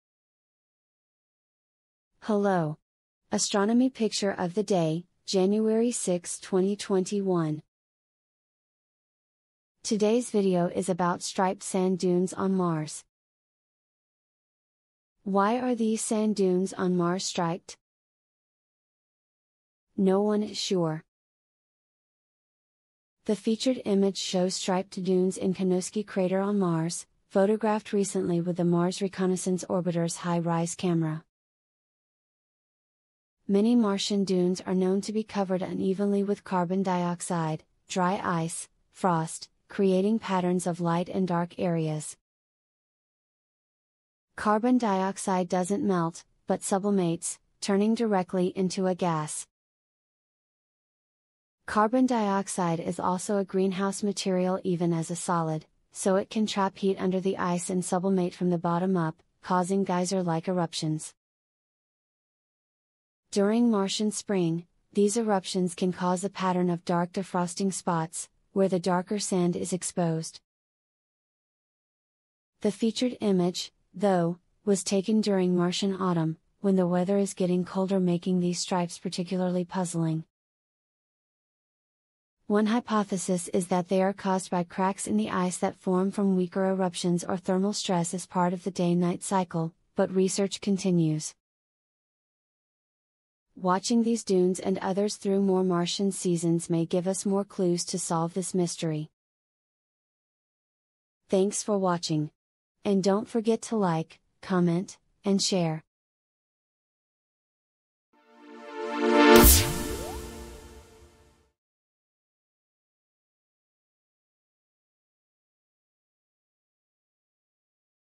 APOD: 2021-01-06 - Striped Sand Dunes on Mars (Narrated by Salli)
Astronomy Picture of the Day - January 6th, 2021 - Striped Sand Dunes on Mars (Narrated by Salli)

Why are these sand dunes on Mars striped?  No one is sure.  The featured image shows striped dunes in Kunowsky Crater on Mars, photographed recently with the Mars Reconnaissance Orbiter’s HiRISE Camera.  Many Martian dunes are known to be covered unevenly with carbon dioxide (dry ice) frost, creating patterns of light and dark areas.  Carbon dioxide doesn’t melt, but sublimates, turning directly into a gas. Carbon dioxide is also a greenhouse material even as a solid, so it can trap heat under the ice and sublimate from the bottom up, causing geyser-like eruptions.  During Martian spring, these eruptions can cause a pattern of dark defrosting spots, where the darker sand is exposed.  The featured image, though, was taken during Martian autumn, when the weather is getting colder – making these stripes particularly puzzling.  One hypothesis is that they are caused by cracks in the ice that form from weaker eruptions or thermal stress as part of the day-night cycle, but research continues.  Watching these dunes and others through more Martian seasons may give us more clues to solve this mystery.